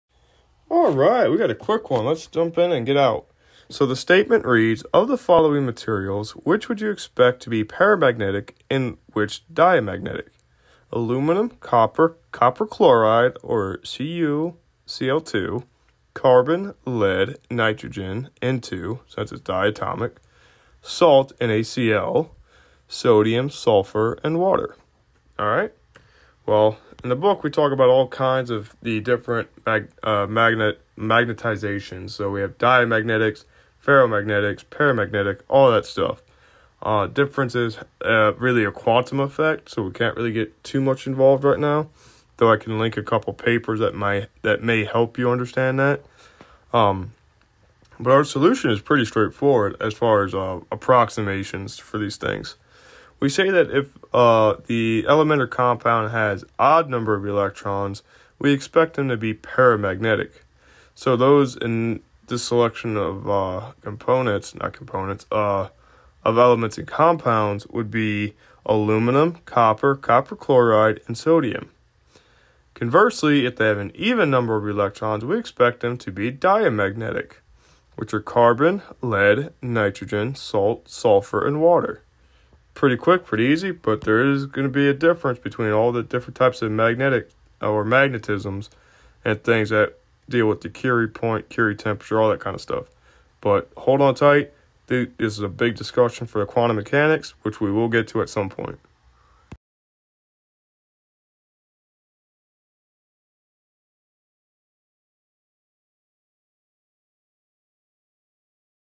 Problem 6.6 - Magnetization, Types of Magnetizations: Introduction to Electrodynamics
I made a post a while back about the different types of magnetizations. I will go find it again and story share it on instagram so you can read through it. But this is where material science comes in and becomes really fascinating. Then of course we see quantum mechanics peaking around the corner.